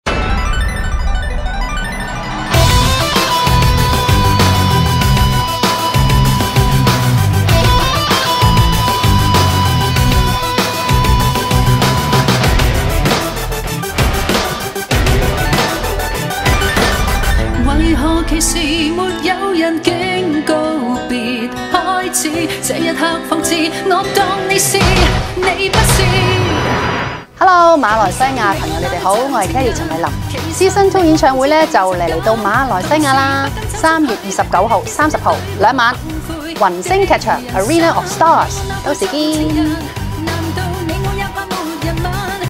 陈慧琳 Season 2 世界巡回演唱会马来西亚站
演唱会以《Season 2》为主题，意味着陈慧琳重新出发，奔赴崭新音乐征程，继续陪伴不离不弃的歌迷迈入人生新阶段。酝酿多时的《Season 2》巡演，由陈慧琳御用的香港赫赫有名音乐人雷颂德担任演唱会总监。首站于去年9月在澳门启动，迅速取得极佳口碑。随后她赴美国、中国、加拿大、香港等地巡演，并将于3月底踏上大马舞台，以最佳状态与好久不见的歌迷见面。

超级宠粉的她，为了回馈歌迷支持，对演唱会的制作高度講究和用心。除了演唱会海報女皇造型相当吸睛之外，她与制作团队精心创新演唱会内容、舞台设计、场面布景、灯光特效、音乐编排、曲目安排、服装搭配及舞蹈配搭，以实现每个环节都带给粉丝群耳目一新的视听感觉。这次的演唱会，她将献唱多首回忆杀经典曲目，包括充满爆发力耳熟能详的舞曲、催泪的抒情歌，以及新歌，带动集体大合唱。